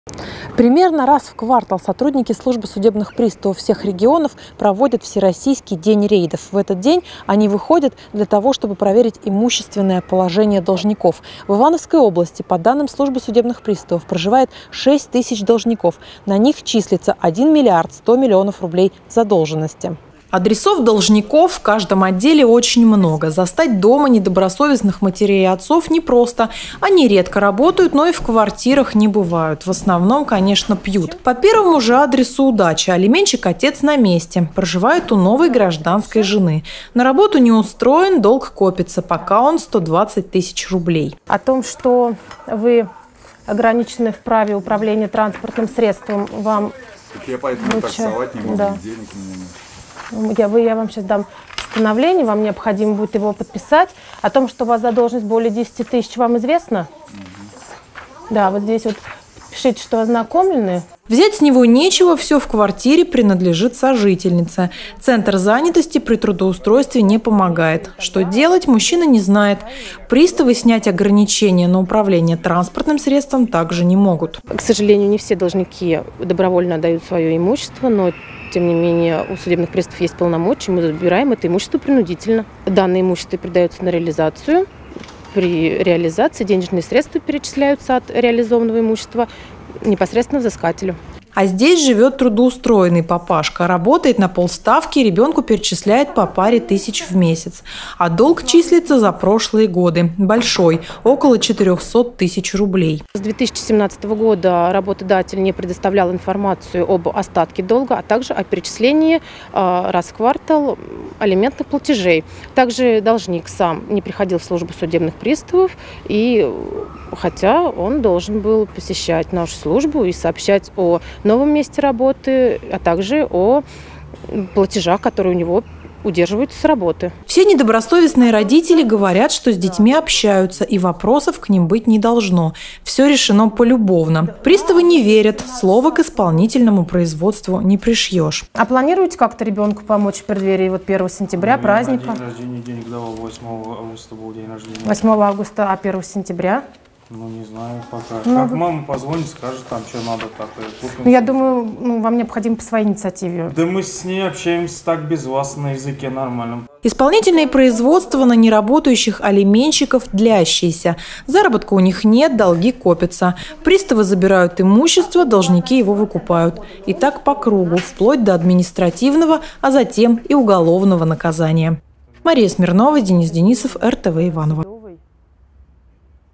Всероссийский рейд по алиментам
Когда родители не хотят добровольно собирать детей в школу,  на помощь приходят судебные приставы.